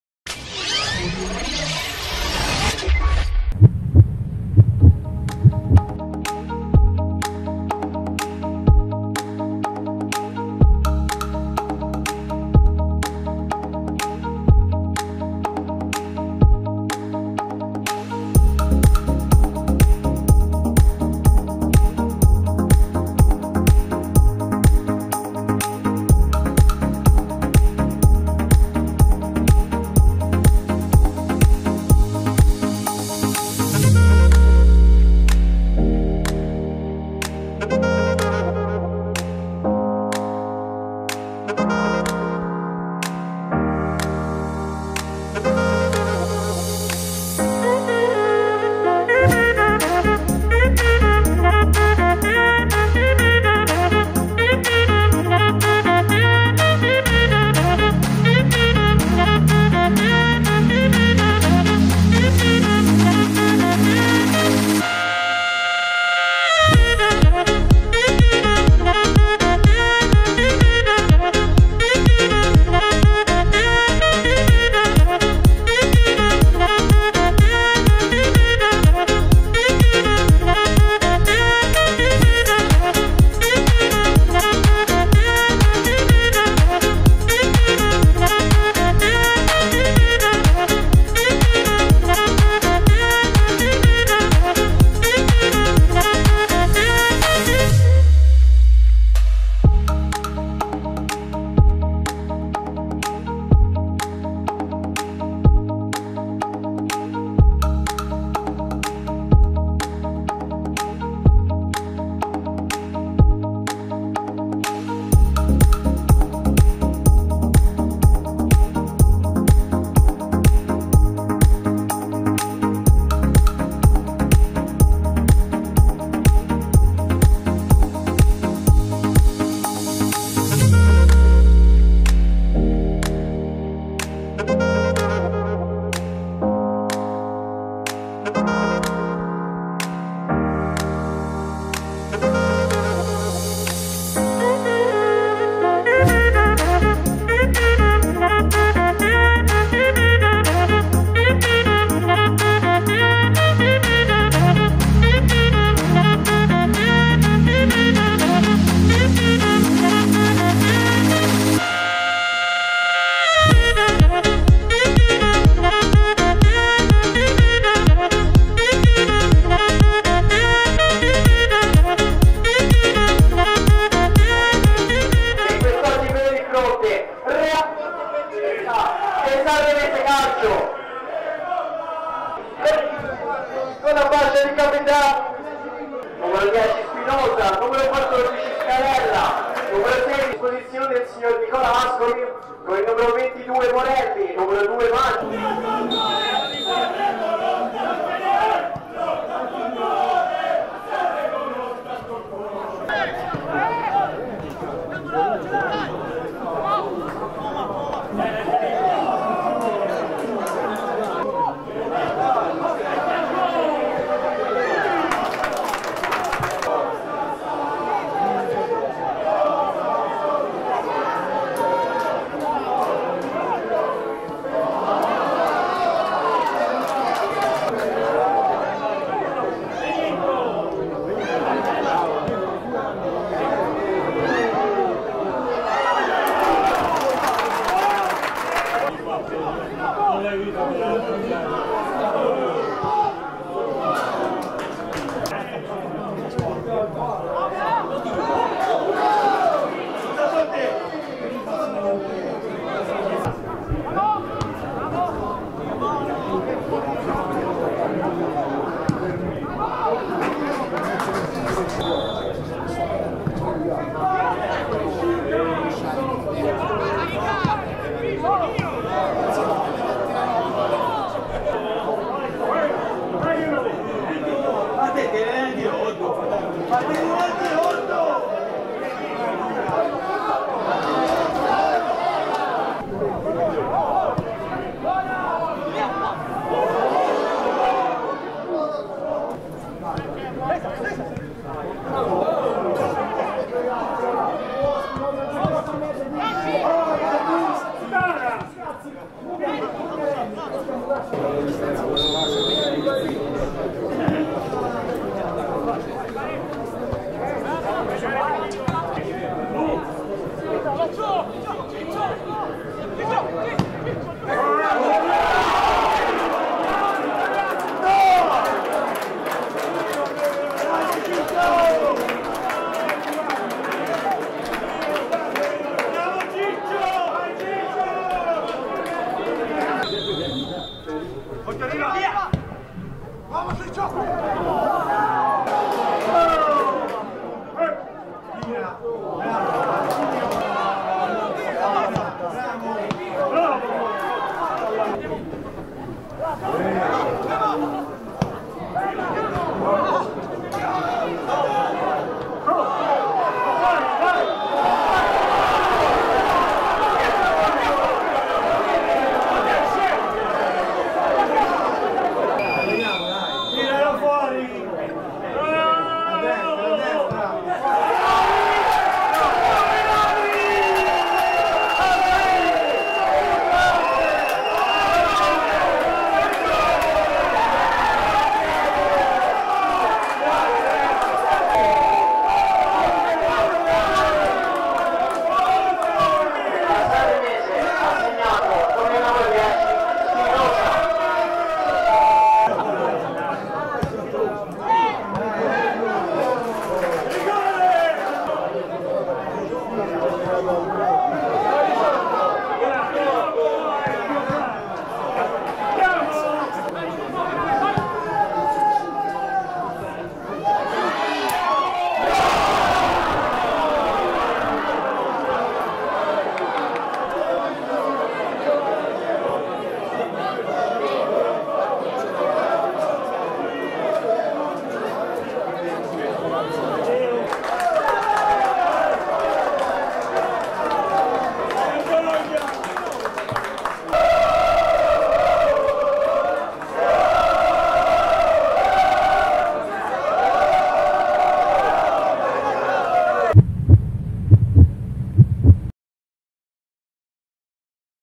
Real Forte Querceta-Sanremese 0-1 13/10/2019
La prima vittoria in trasferta in campionato dei biancoazzurri nelle immagini del nostro Nico Cosentino